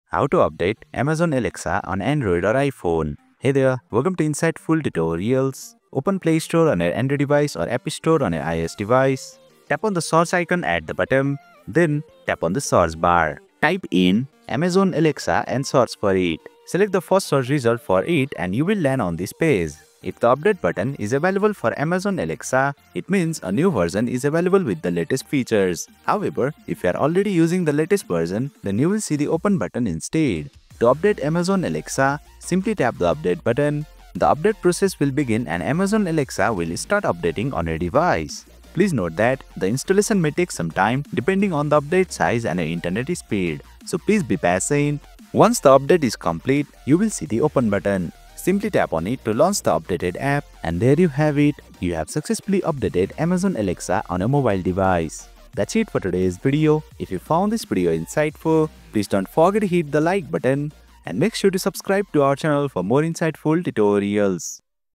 How To Update Amazon Alexa on Android/iPhone (2025)
Is your Amazon Alexa app not working properly or missing the latest features? In this video, I’ll guide you step-by-step on how to update Amazon Alexa on Android and iPhone in 2025. Whether you're using an Android phone or an iPhone, you’ll learn exactly how to get the latest Alexa app update from the Play Store or App Store—even if you're outside the US.

🔹 Tips:
✔️ Updating the Alexa app ensures compatibility with your smart devices.
🌐 If you're outside the US, make sure to switch your app store region settings first.
📱 Works for both Android and iOS.

⏳ Timestamps:
00:00 - Intro
00:10 - How to Update Amazon Alexa on Android/iPhone
00:30 - Updating Amazon Alexa on Android/iPhone

Stay updated with helpful guides and mobile how-to tutorials you can trust.